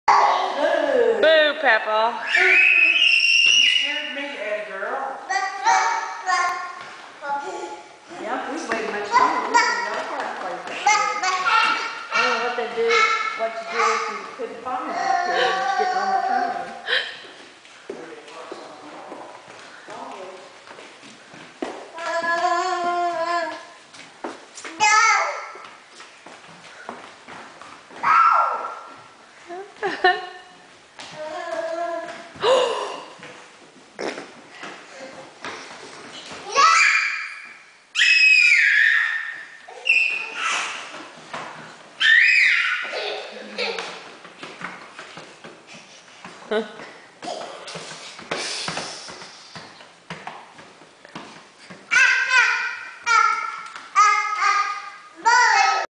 Addy
Addy running around the train station in Harpers Ferry, WV.